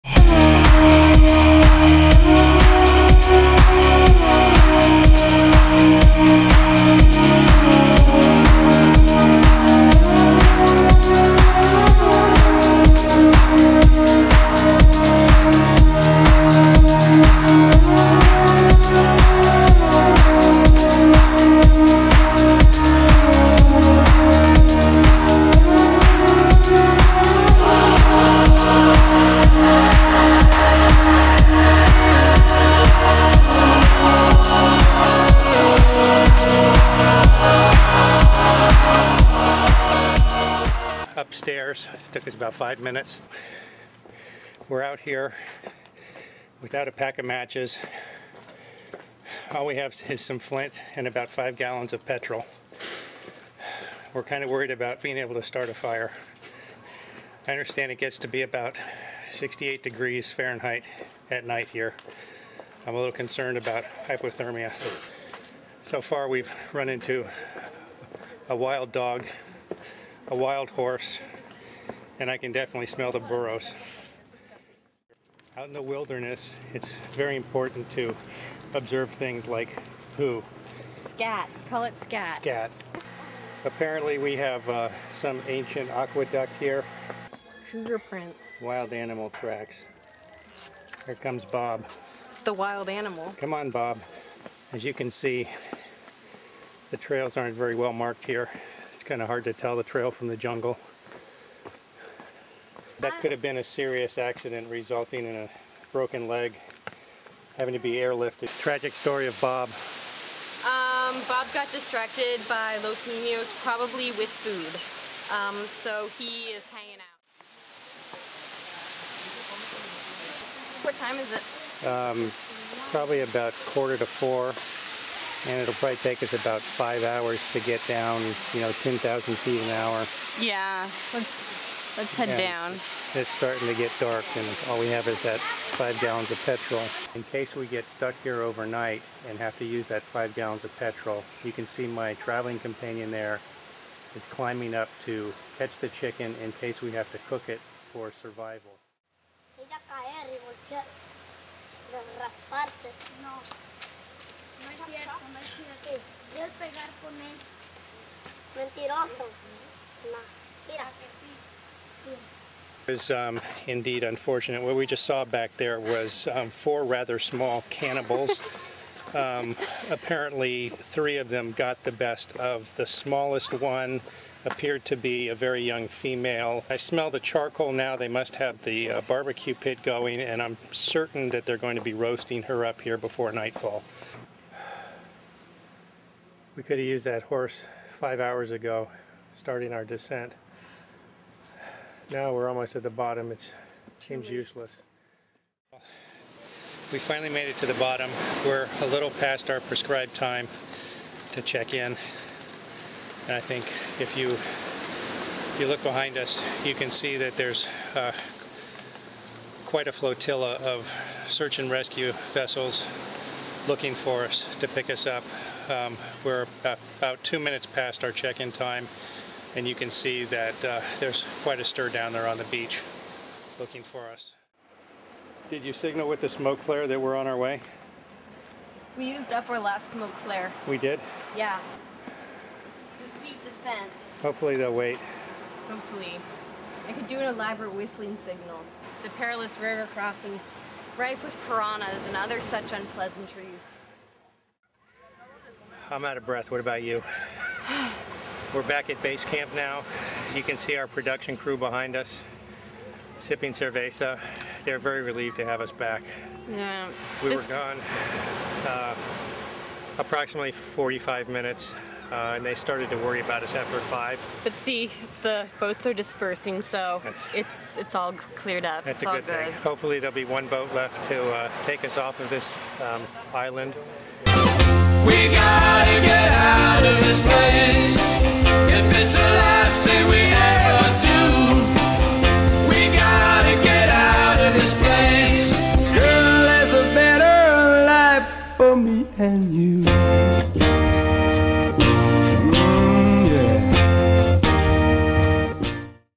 Survivor Man - Mexico
Parody of the popular TV series. Shot in Yelapa Mexico, the duo searches for cascading falls deep in the jungle. During their journey they encounter many dangers that will test their skills. Bob, a less than loyal basset hound they meet along the way decides perhaps he made a mistake by tagging along.